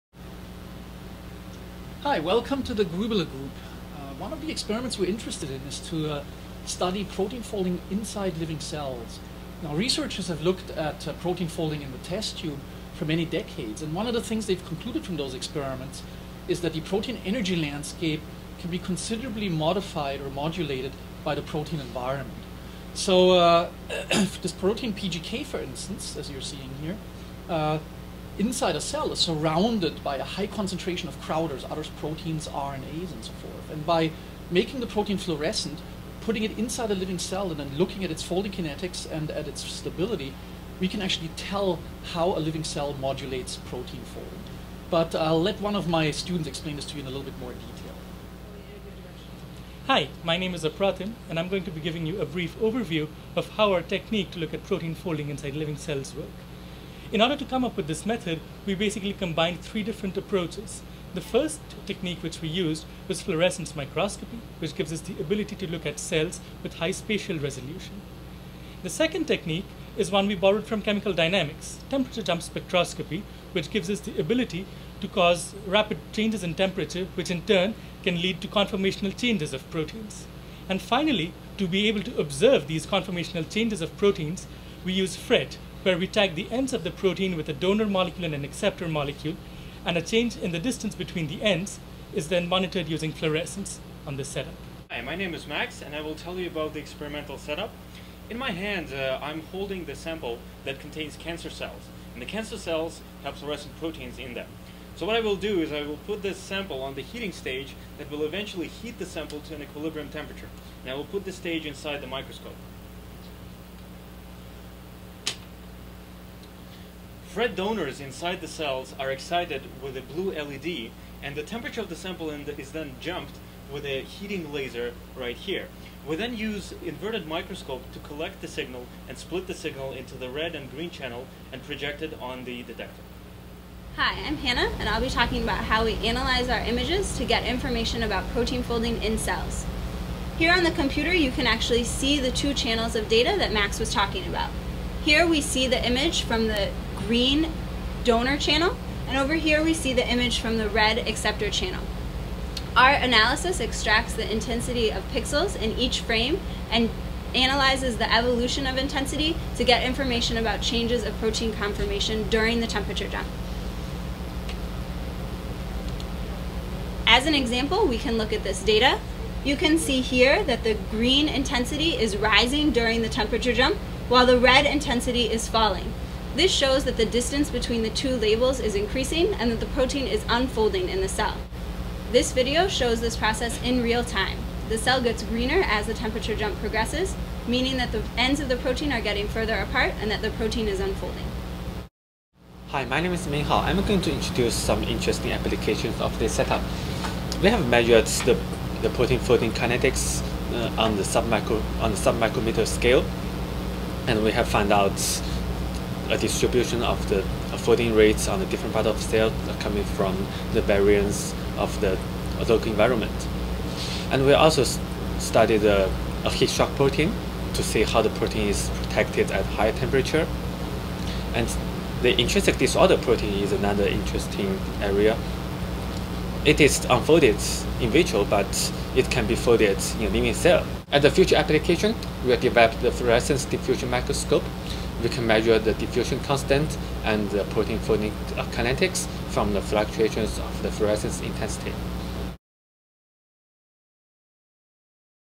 Gruebele Lab, School of Chemical Sciences at the University of Illinois
Featuring Martin Gruebele, Apratim Dhar, Max Prigozhin, Hannah Gelman, and Minghao Guo

Tour and description of Fast Relaxation Imaging (FReI) featuring members of the Gruebele Lab at the University of Illinois,  Urbana-Champaign.